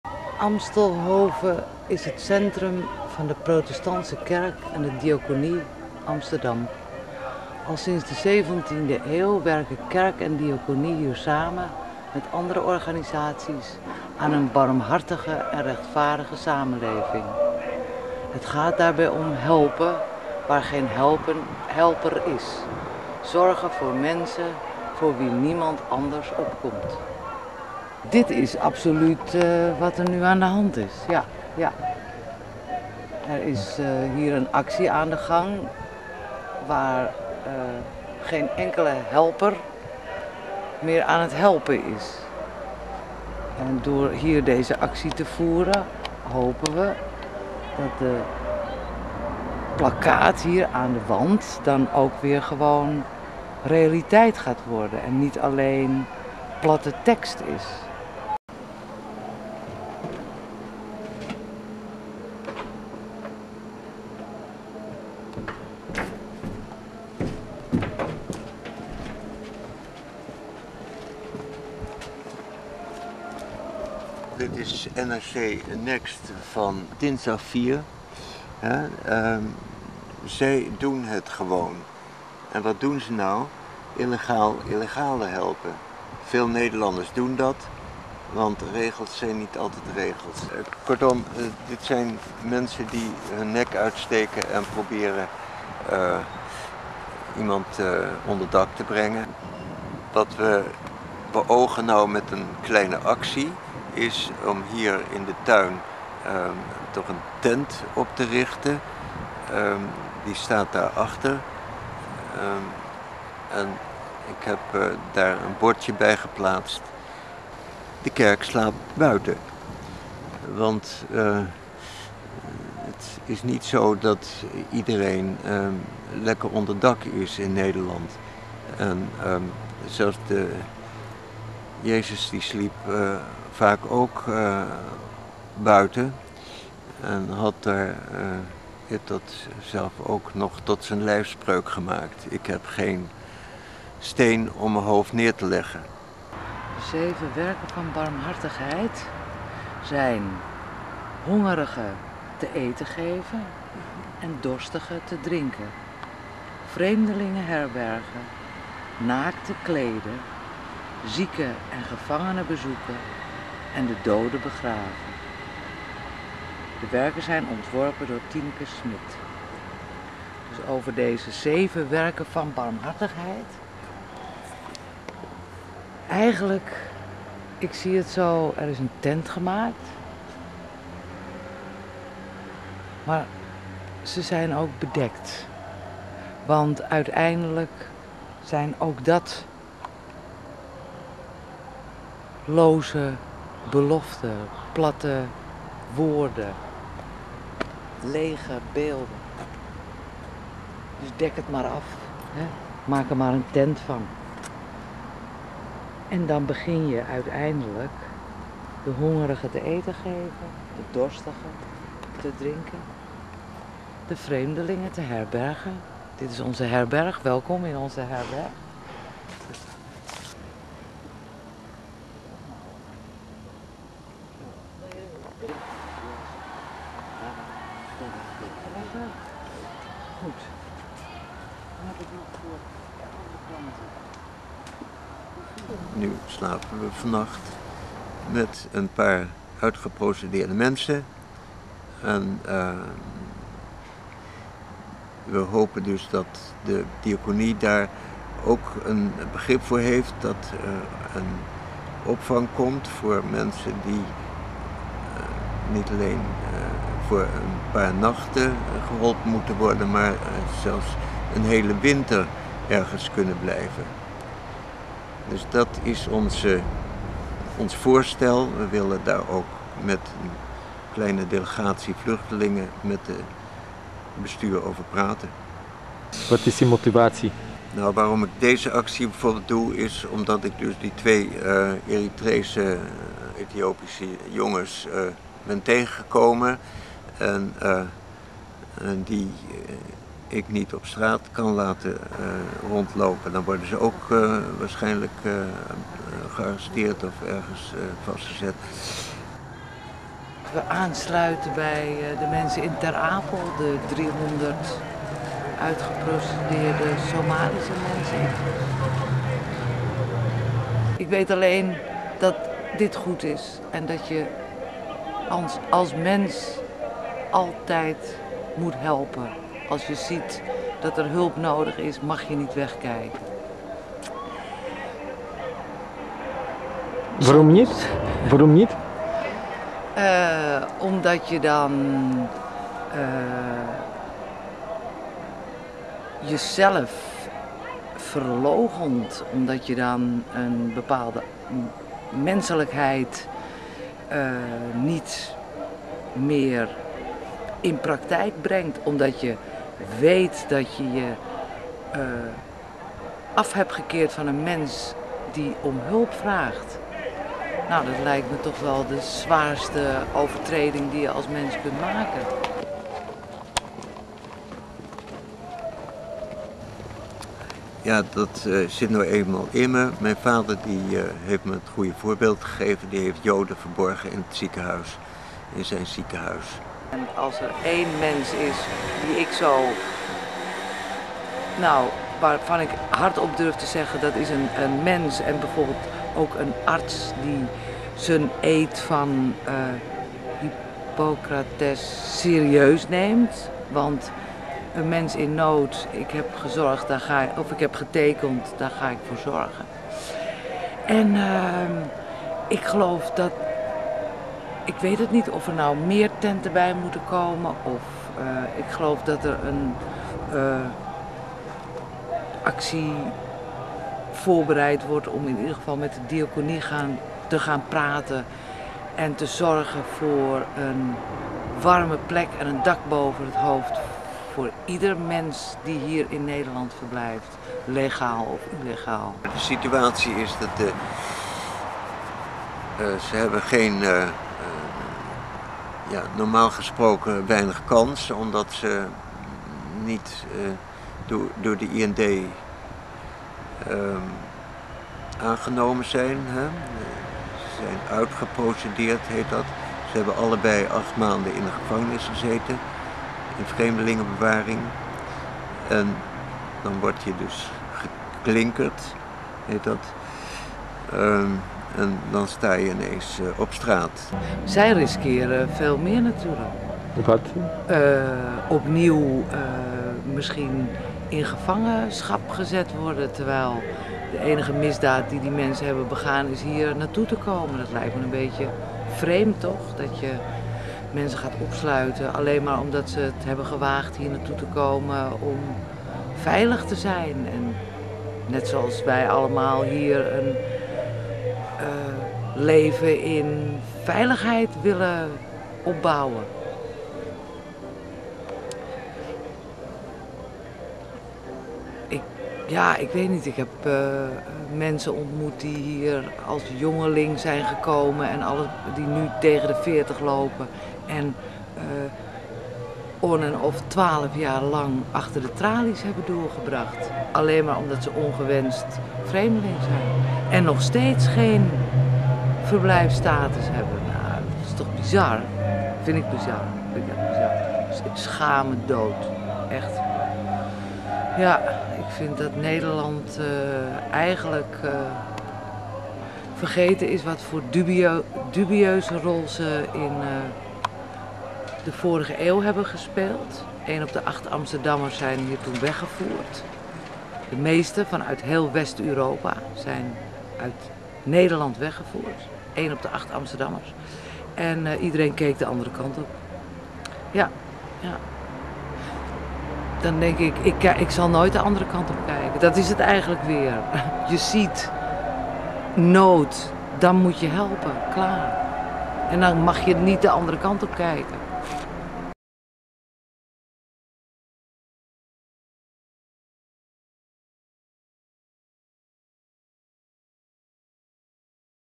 Occupy Amstelhoven.Lange versie
Yesterday night group of activist and refugees on street occupied terrain of protestant church to convince them to be honest to their words and give shelter to them.
De kerk slaapt buiten Zo heeft dokter Co zijn actie gedoopt. Co is de blote voeten dokter
In de tuin vsn het Wereldhuis is dinsdagavond 4 september een zeil vsn 10 x 8 meter gedrapeerd over de 7 beelden die staan voor de 7 werken van barmhartigheid. Hieronder slapen vluchtelingen op straat. De eerste drie komen uit de Hoorn van Afrika en slapen al maanden op straat. Doel van de aktie is om zoveel mogelijk vluchtelingen onder dak te brengen. Voor de winter.
We zijn benieuwd hoe Paul van Oosten, het hoofd van de armenzorg, gaat reageren op de actie in zijn tuin.